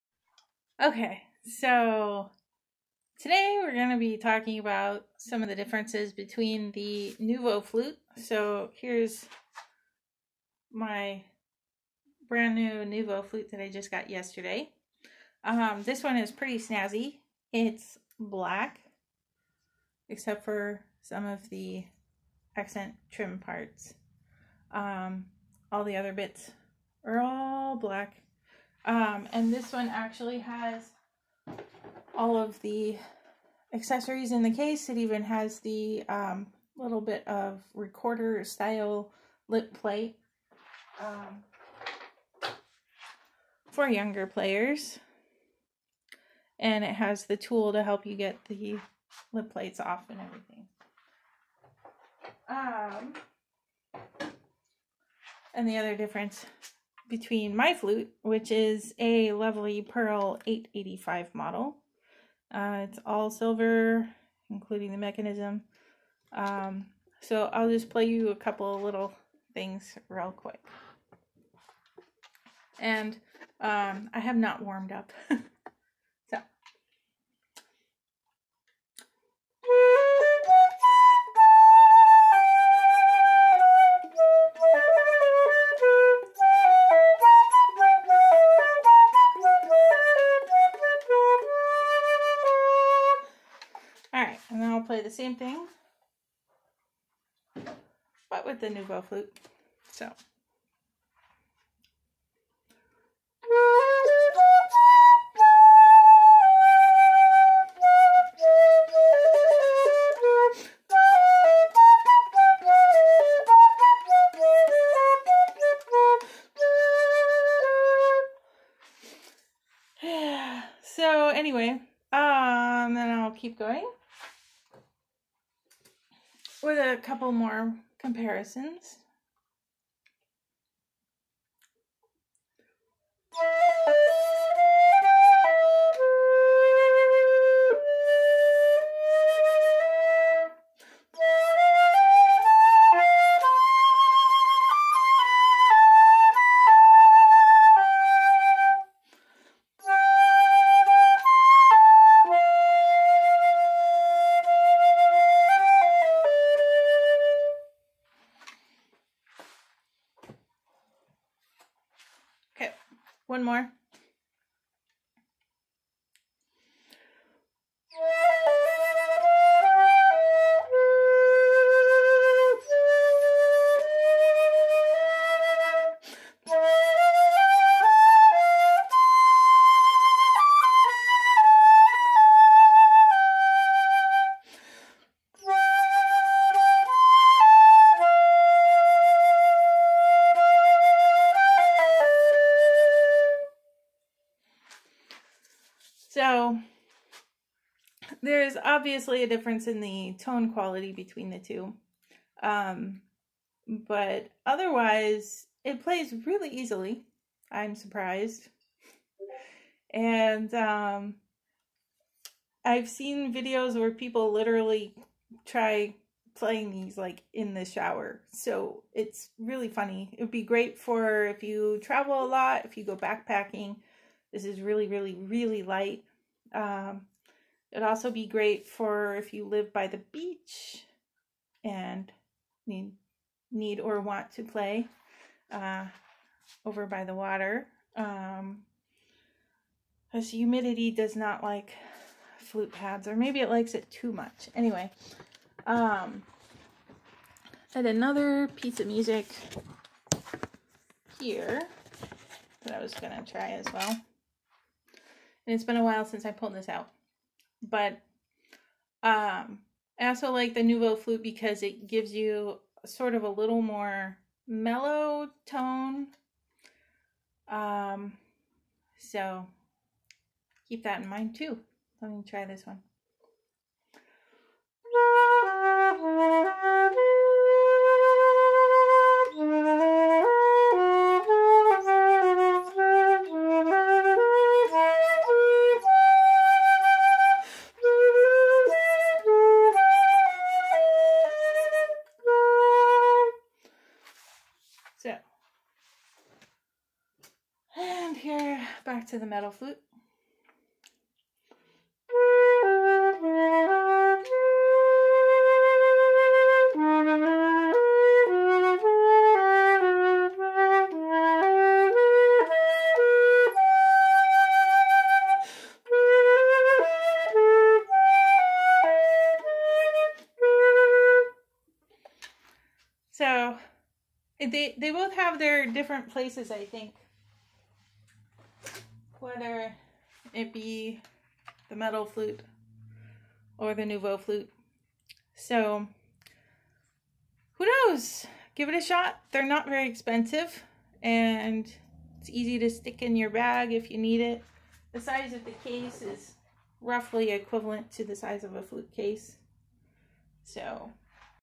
Short Nuvo Flute Comparison
A short comparison of my flute to a Nuvo flute.

Short answer: I like both!

I am keeping a different color Nuvo flute and selling this black one. It is a fun instrument, and I hope it finds a good home.